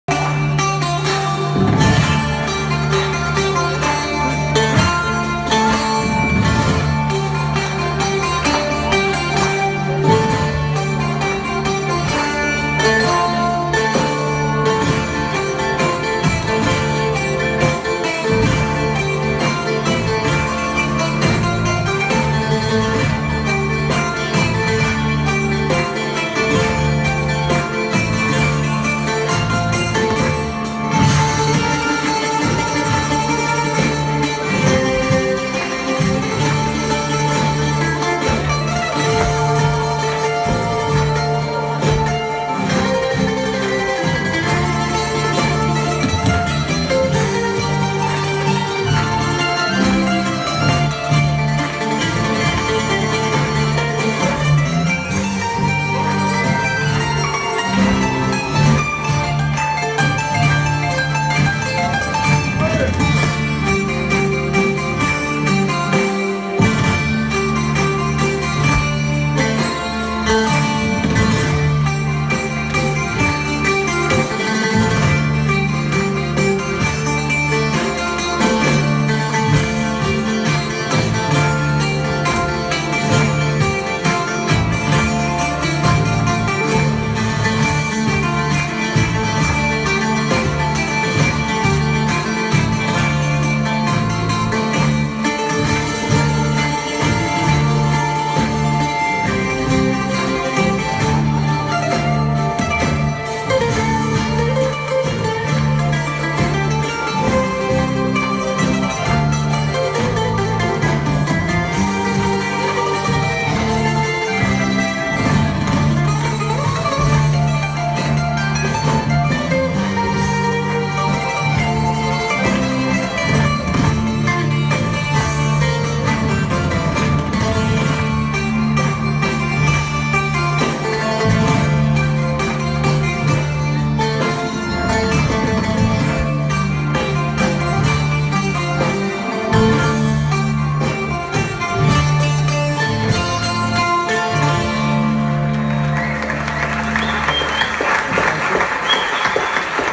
Ο ΑΝΤΩΝΗΣ ΧΟΡΕΥΕΙ ΖΕΙΜΠΕΚΙΚΟ ΤΟΥ ΔΗΜΗΤΡΗ ΠΕΤΡΟΠΟΥΛΟΥ!